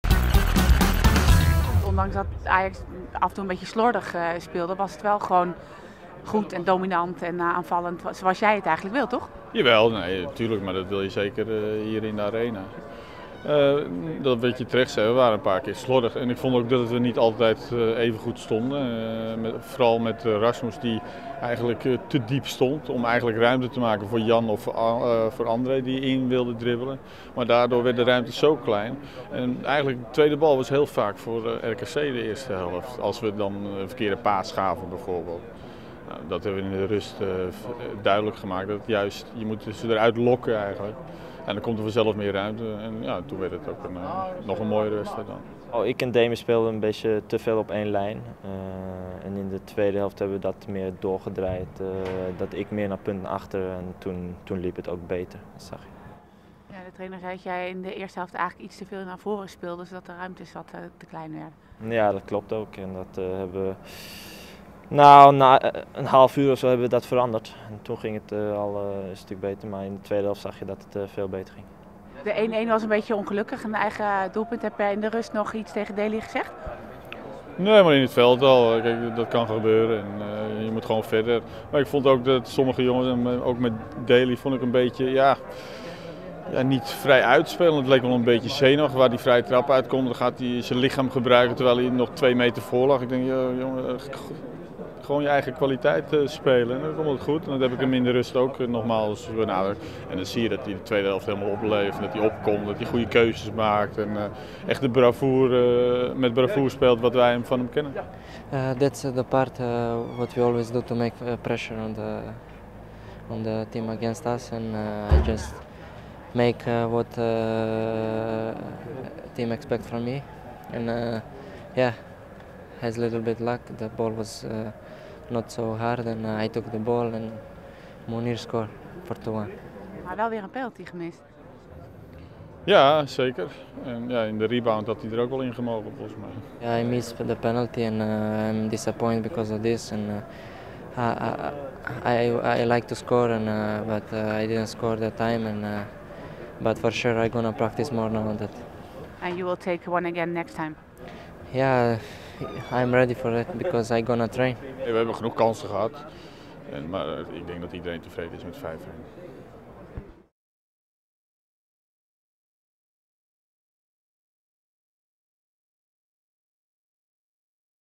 De Boer: 'Tevreden met 5-1'
Ajax bereikte donderdagavond de finale van de KNVB Beker dankzij een ruime zege op RKC. Trainer Frank de Boer, Rasmus Lindgren en Miralem Sulejmani geven hun visie op de zege.